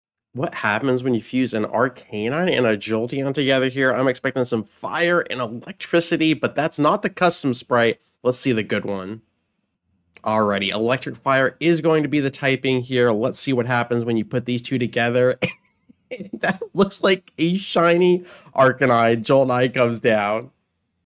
When an Arcanine loves a Jolteon...
Have you ever wondered what an Arcanine and Jolteon fused together might look like? Well...